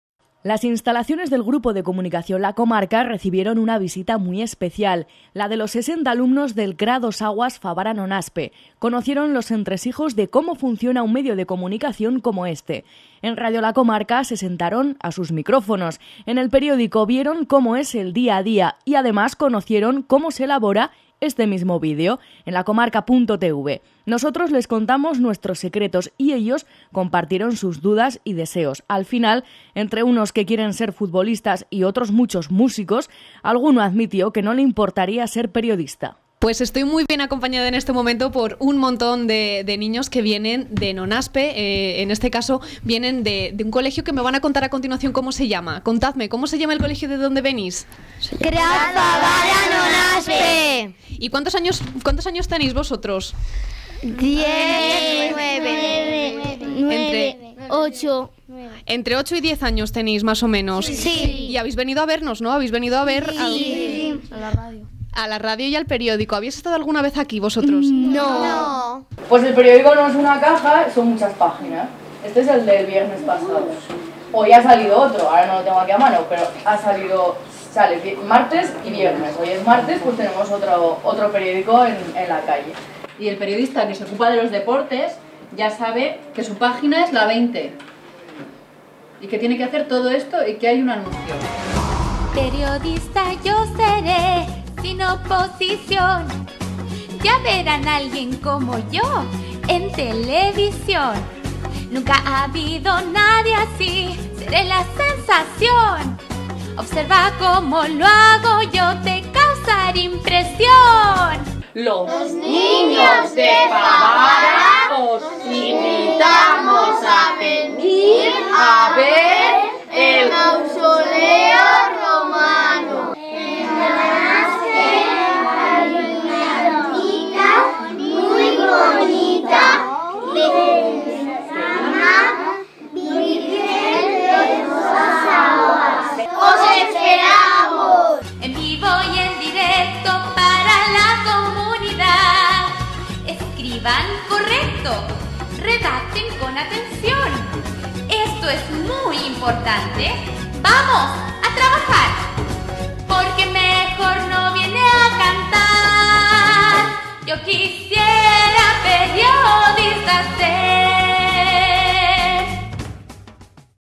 La Comarca.tv - Visita colegios Fabara y Nonaspe a La COMARCA
Unos 60 niños del CRA de Fabara y Nonaspe visitaron el martes 6 de mayo los estudios de radio La COMARCA y la redacción del periódico. Hicieron preguntas y alguno se animó a ser de mayor presentador de televisión, locutor de radio o periodista.